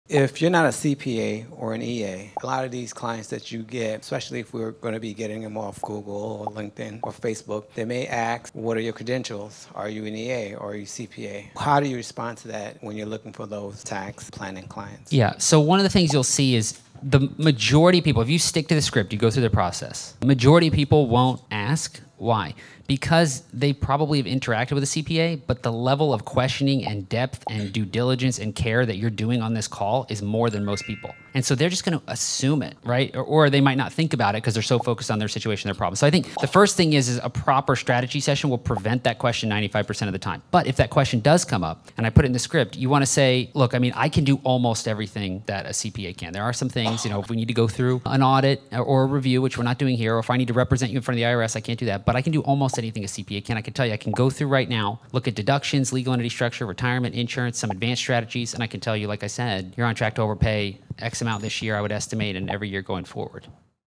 Are Credentials Everything? Discover the Untapped Potential of Tax Planning!
Discover effective strategies for addressing credential inquiries from potential tax planning clients, ensuring their confidence in your capabilities as you navigate the strategies to help them optimize their finances.

Save Money On Taxes Today